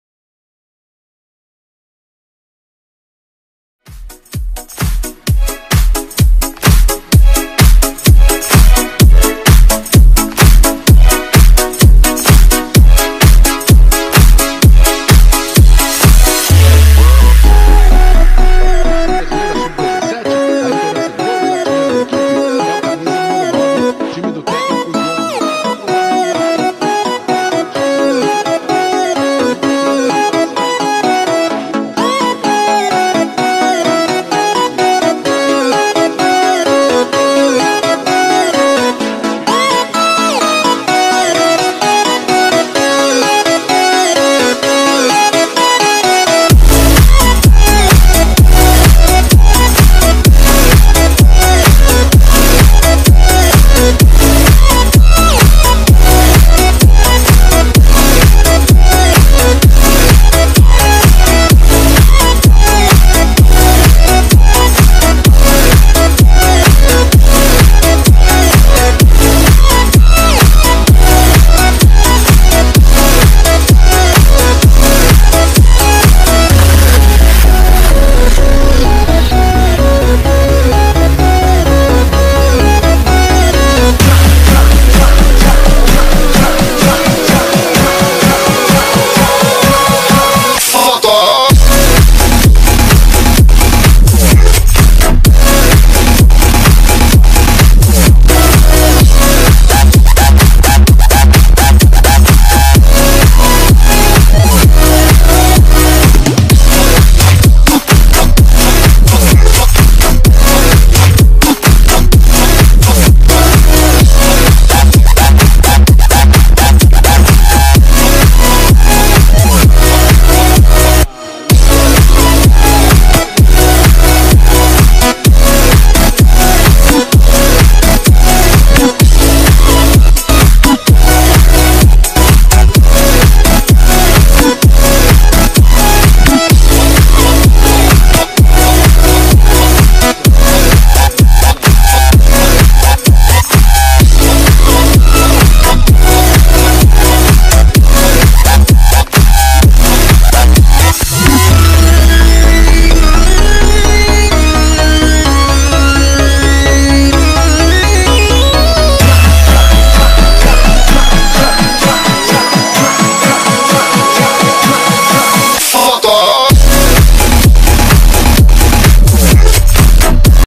Alunos de Amaral fazendo bagunça(82)
Eles sao da escolinha de futebol craques do futuro professor Amaral